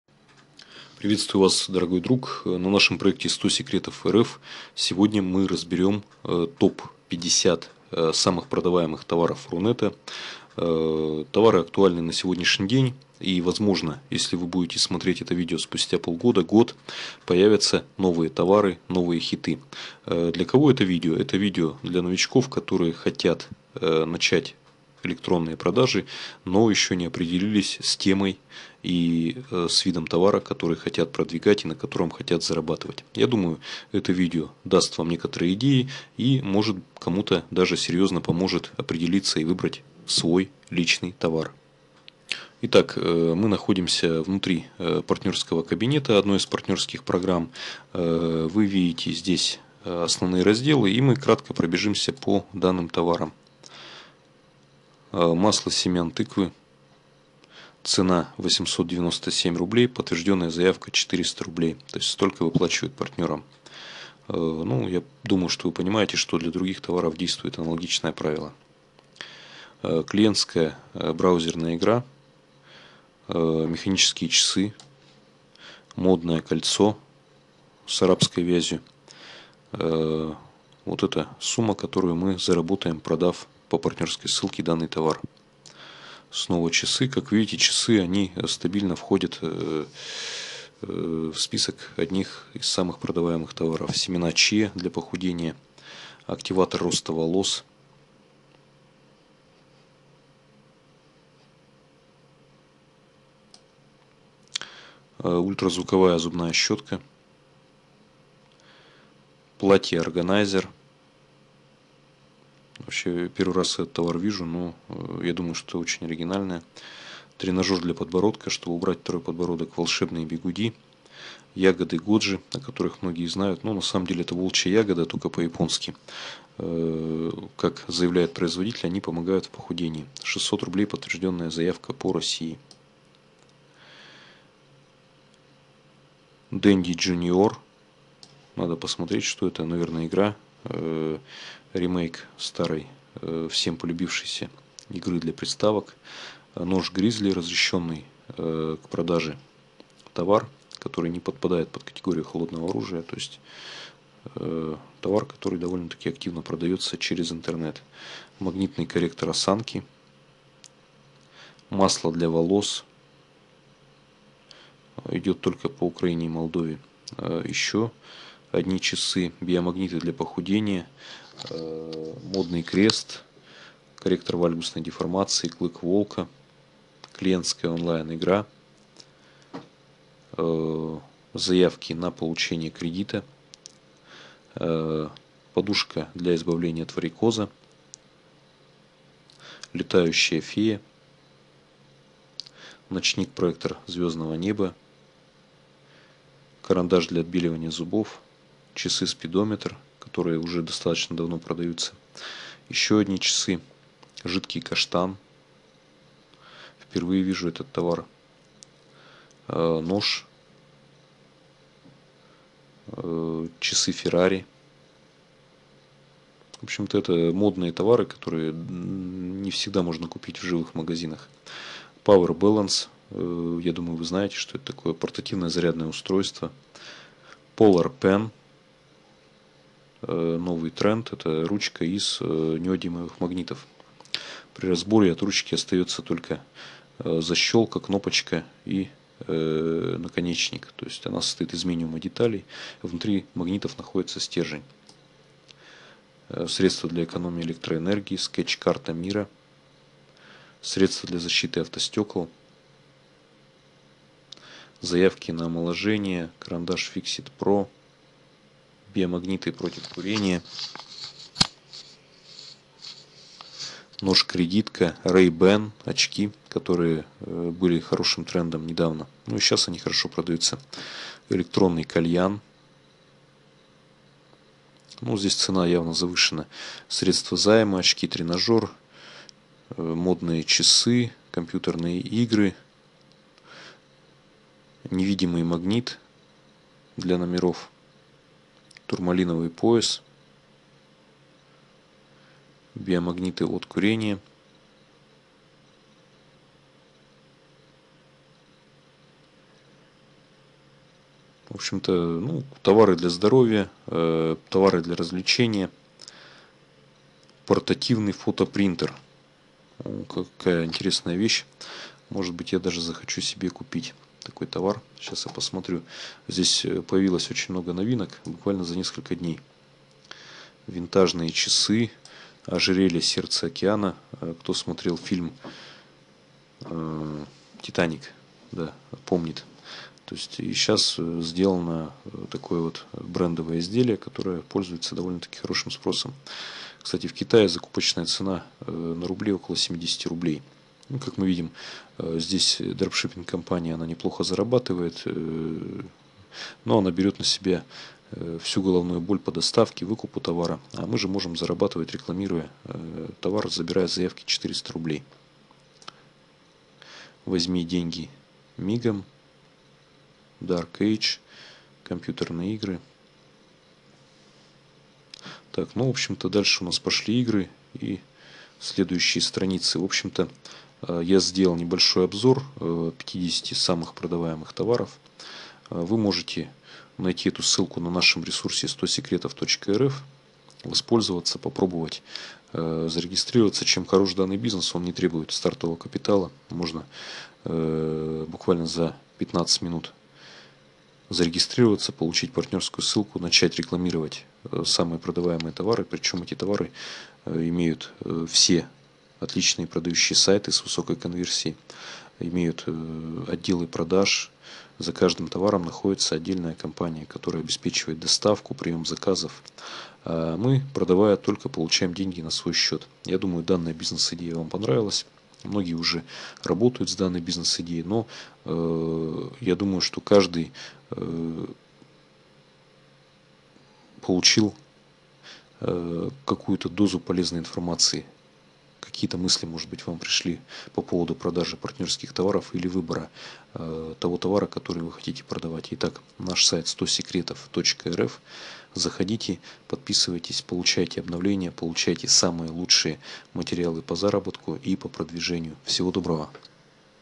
30 Бизнес кейс 50 товаров хитов продаж Рунета 2014. Дропшипинг отдыхает)
В этом обзоре есть и сами товары. И конечно же мы не обошли внимание как с ними работают партнеры. А они неплохо зарабатывают ведь партнерский процент часто больше 50%